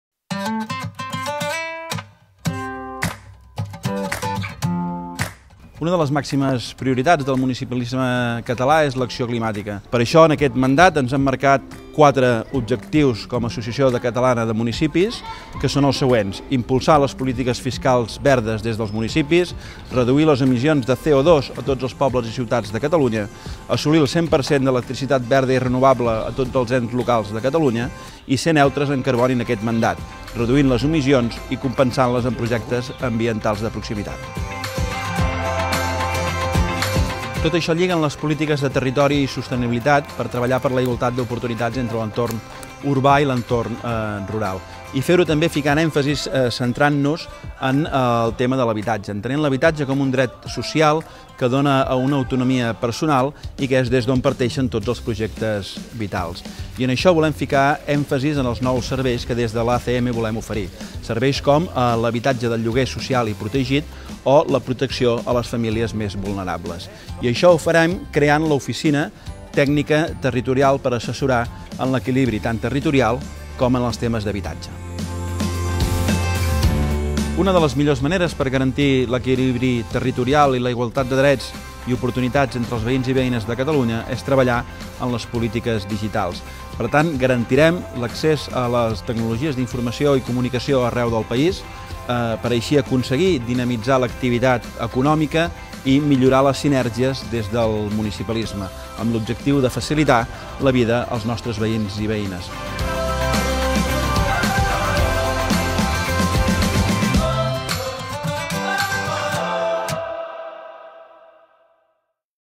Territori Digital i Sostenibilitat
🌳📶🏠 L'Acció Climàtica, les Polítiques Digitals i l'Habitatge són 3 de les màximes prioritats de l'agenda municipalista.

🔧 Donem eines als pobles i ciutats per descarbonitzar la mobilitat, com ara amb l’adquisició de vehicles híbrids i elèctrics, reduir la petjada de carboni, la transició energètica, subministrant energia 100% verda i renovable al món local, o protegint la biodiversitat, mitjançant els Objectius de Desenvolupament Sostenible o l’Agenda Rural.

☑️ Impulsem les polítiques fiscals verdes des dels municipis en matèria de residus, l’autoconsum energètic i afavorim la mobilitat sostenible.